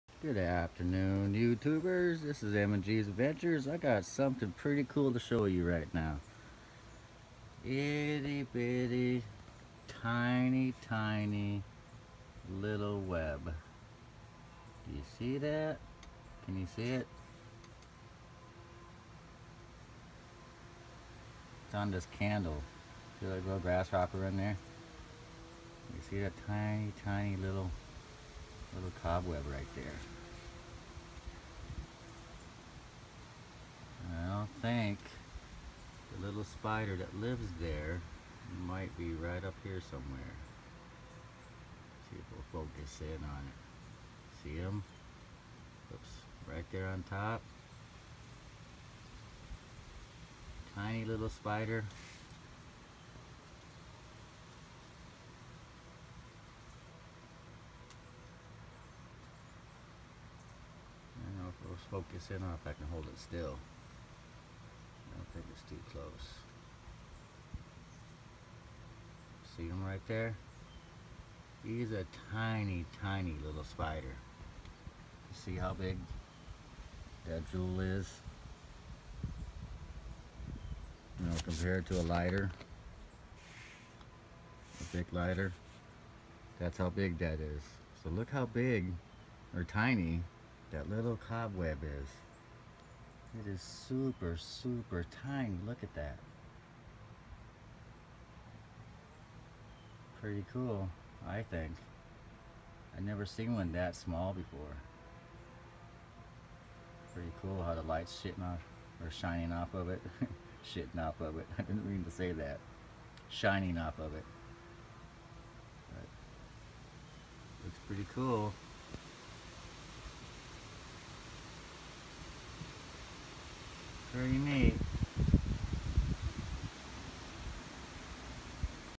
Tiny spider web
I found a tiny web almost intact. Very cool to see nature as tiny and magnificent as this.